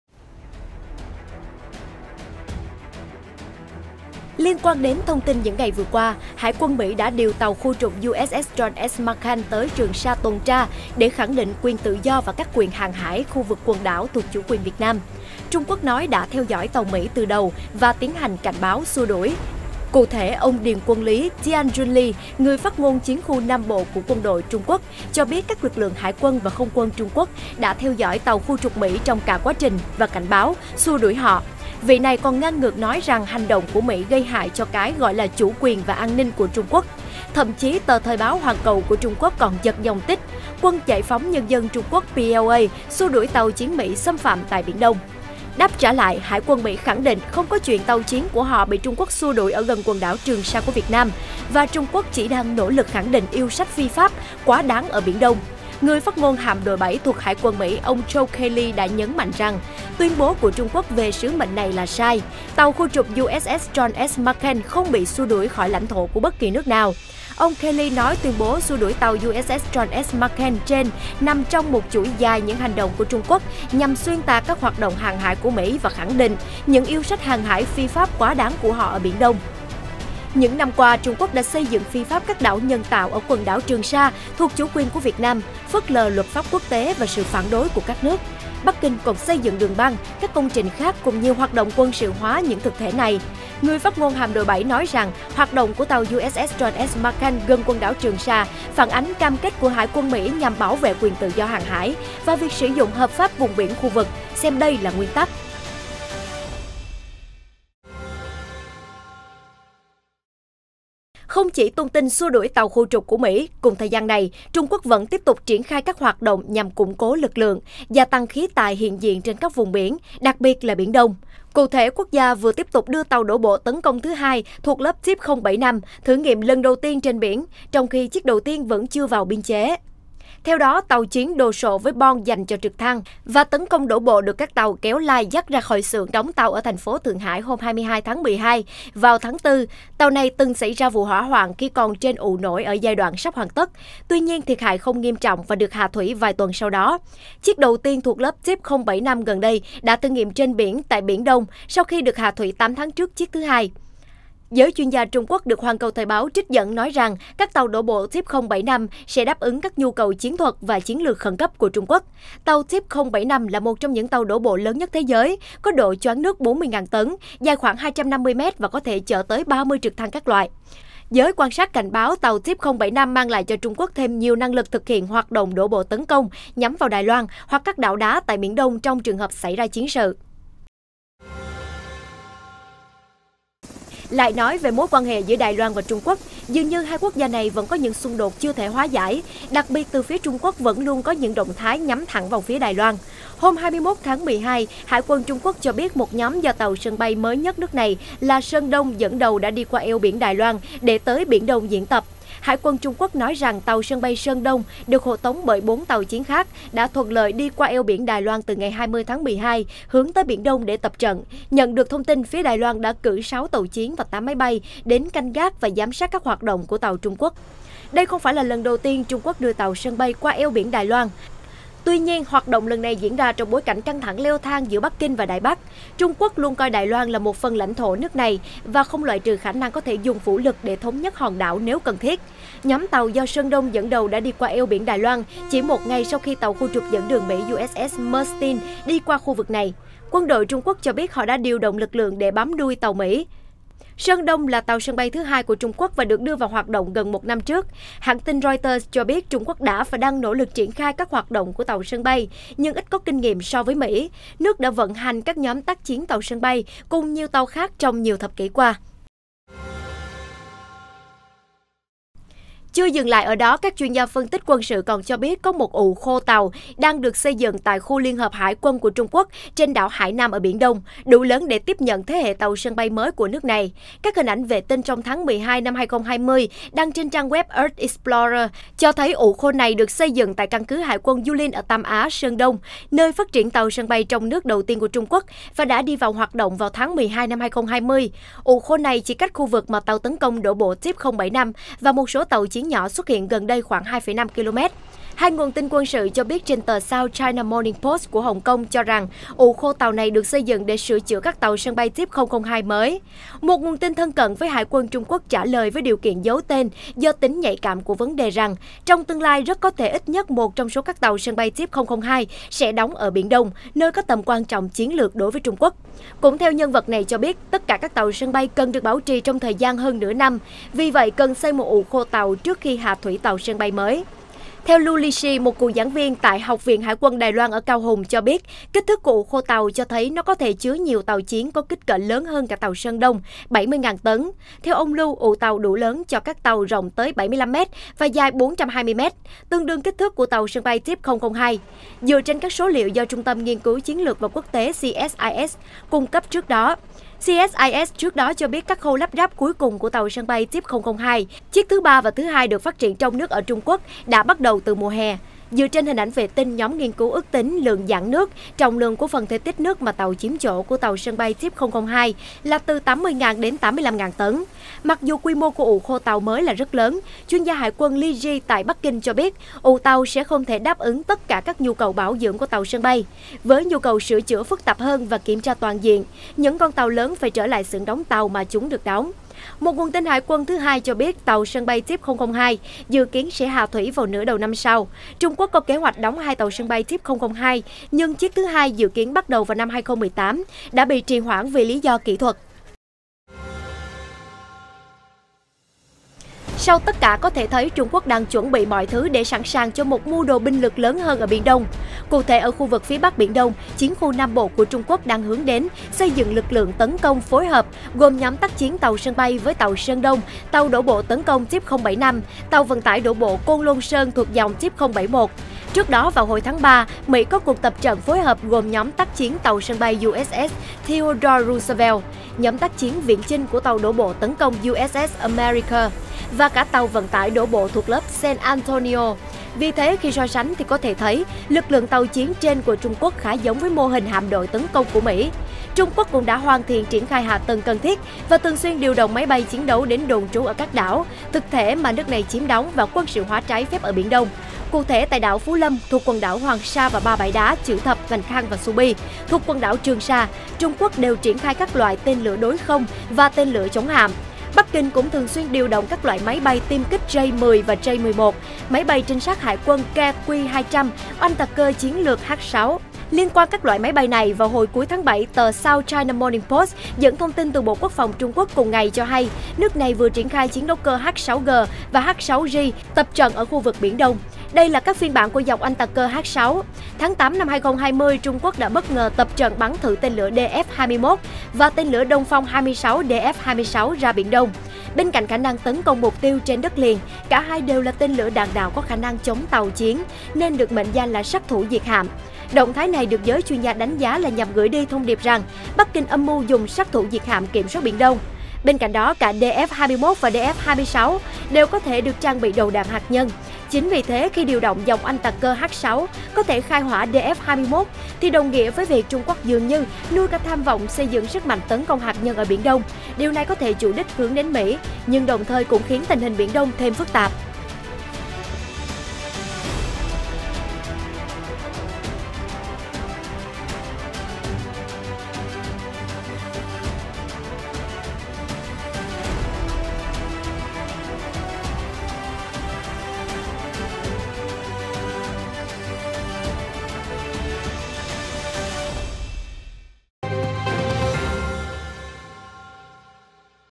Tin Biển Đông | Trung Quốc triển khai binh lực cho mưu đồ mới ở Biển Đông? | FBNC
Tin Biển Đông | Trung Quốc triển khai binh lực cho mưu đồ mới ở Biển Đông?
Trung Quốc đang chuẩn bị mọi thứ để sẵn sàng cho một mưu đồ binh lực lớn hơn ở Biển Đông. Trung Quốc cũng đã hoàn thiện triển khai hạ tầng cần thiết và thường xuyên điều động máy bay chiến đấu đến đồn trú ở các đảo, thực thể mà nước này chiếm đóng và quân sự hóa trái phép ở Biển Đông.
Chưa dừng lại ở đó, các chuyên gia phân tích quân sự còn cho biết có một ụ khô tàu đang được xây dựng tại khu liên hợp hải quân của Trung Quốc trên đảo Hải Nam ở Biển Đông, đủ lớn để tiếp nhận thế hệ tàu sân bay mới của nước này.

FBNC
ĐỒNG TIỀN THÔNG MINH - CUỘC SỐNG THÔNG MINH